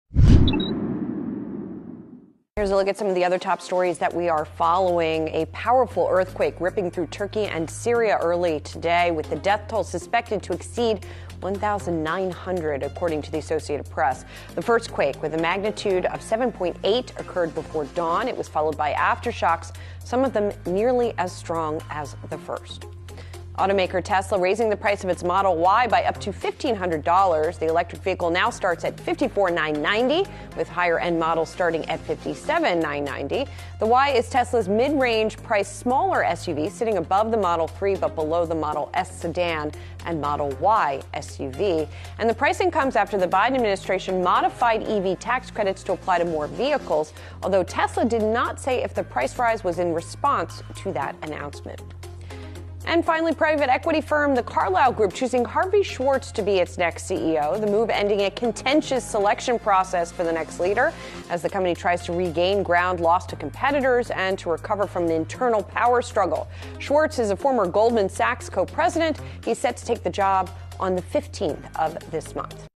Earthquake hits Turkey and Syria, Tesla raises Model Y prices, Carlyle names Harvey Schwartz CEO
Yahoo Finance Julie Hyman reports the latest notable business headlines including Turkey and Syria being hit by a 7.8 magnitude earthquake, Tesla raising its Model Y prices by as much as $1,500, and the Carlyle Group choosing Harvey Schwartz as the company’s new CEO.